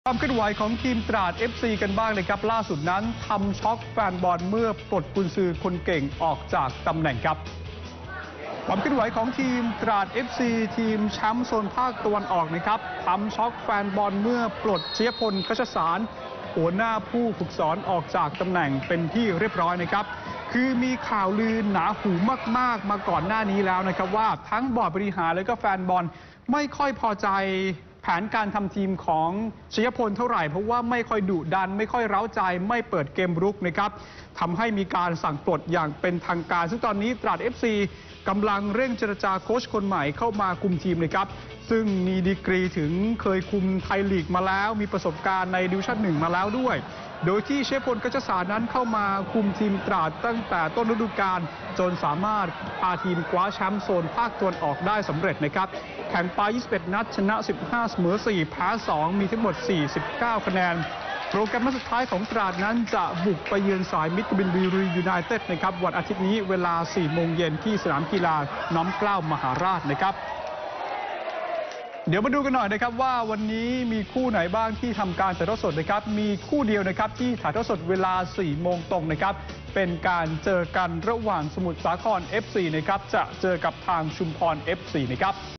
"ตราด" ปลด "ชยพล" เหตุรูปเกมไม่เร้าใจ
พิกัดกีฬา ช่วงเช้า : ฟุตบอลดิวิชั่น 2 "ตราด" ปลด "ชยพล" เหตุรูปเกมไม่เร้าใจ "NOW26" 4-9-59